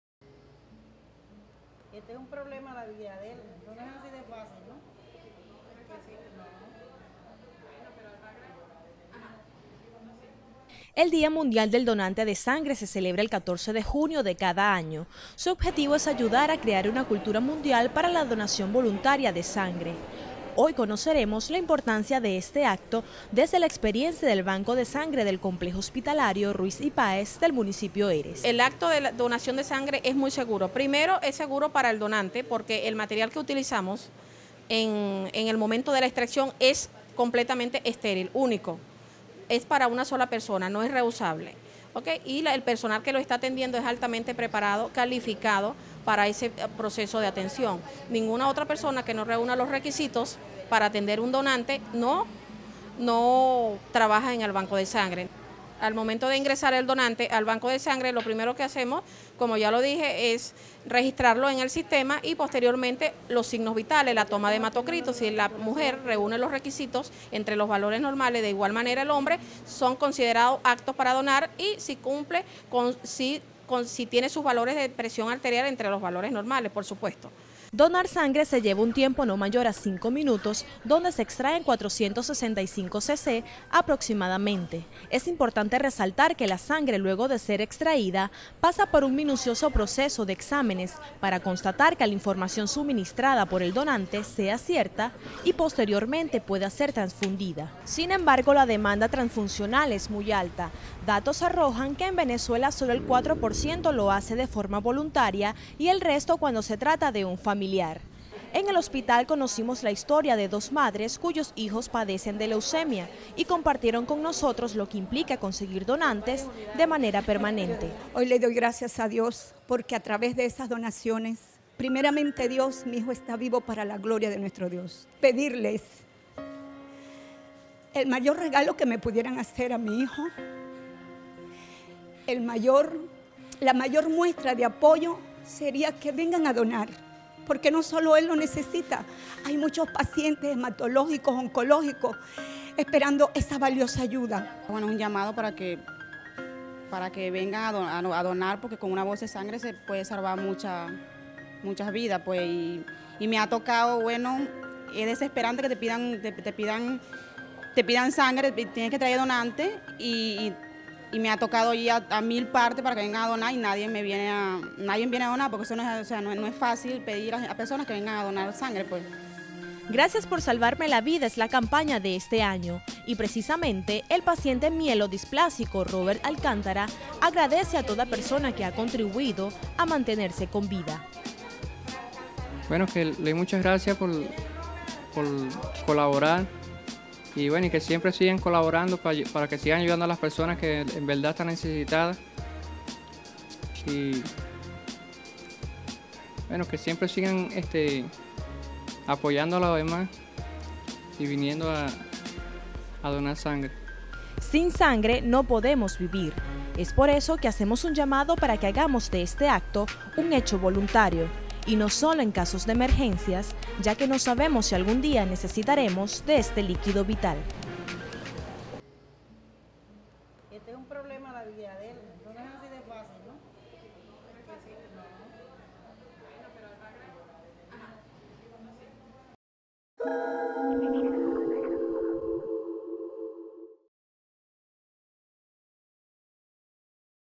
ESPECIAL | Donar sangre: Salvar vidas
Para sensibilizar a la población acerca de la importancia de la donación de sangre voluntaria, se lleva cabo una jornada de promoción en el Banco de Sangre del Hospital Universitario Ruíz y Páez en Ciudad Bolívar.